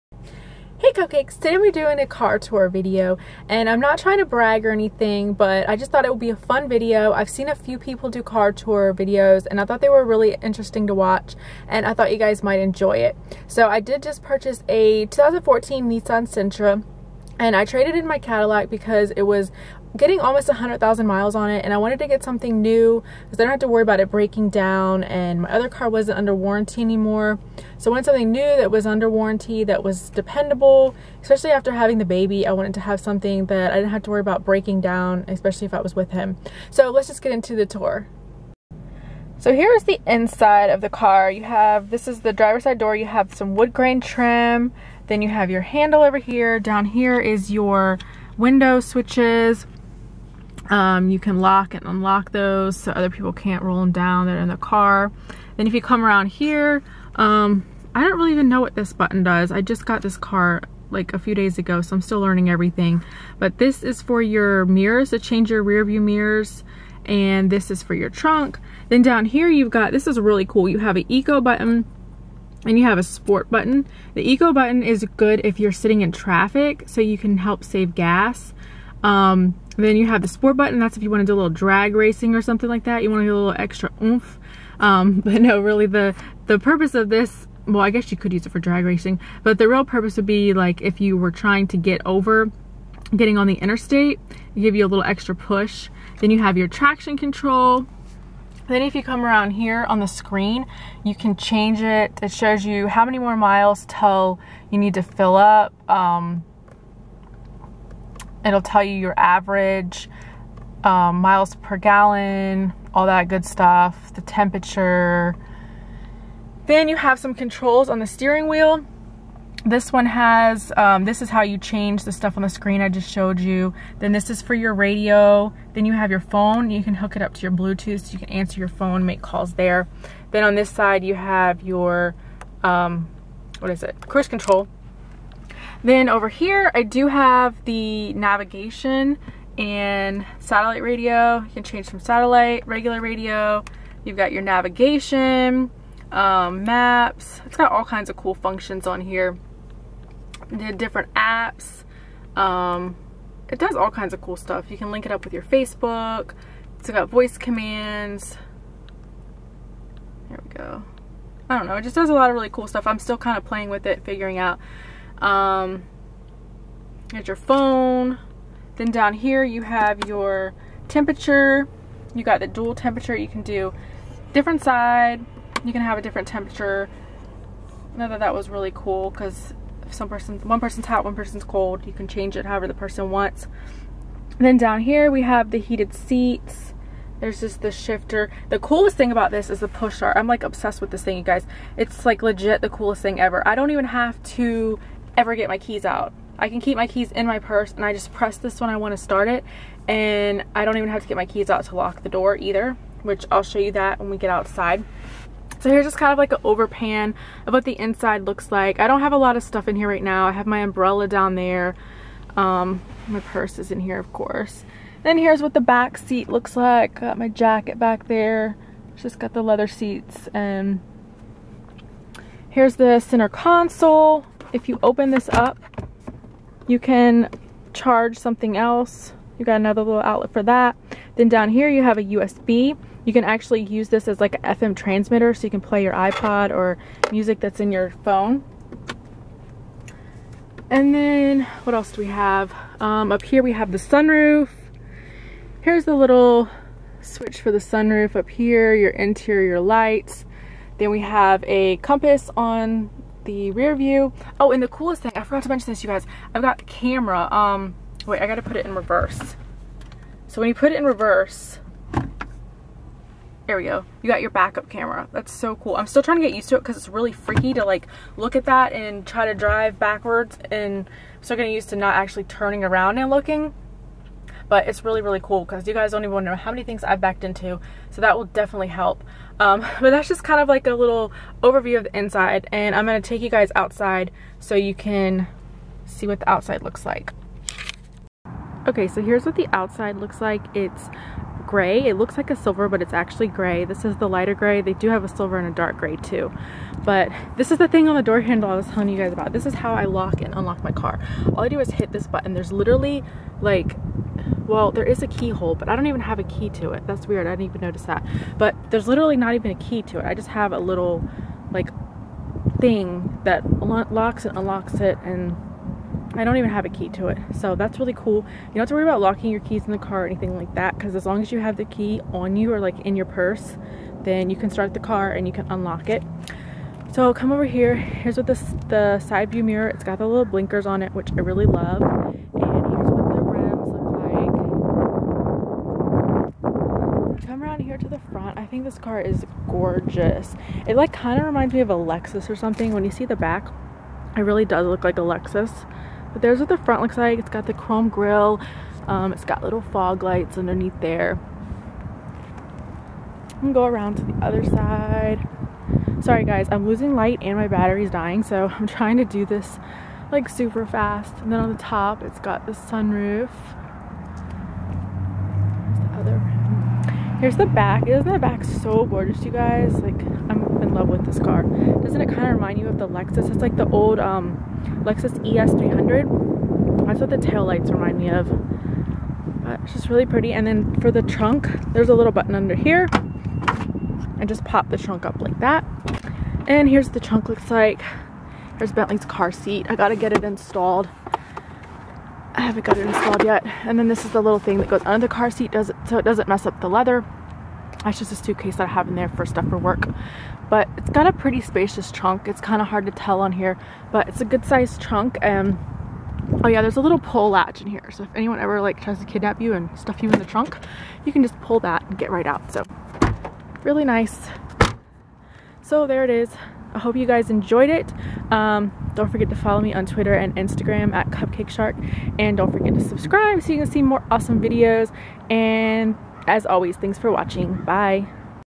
Car Tour
Just a quick little vlog to show you my new car. It’s a 2014 Nissan Sentra and Im loving it!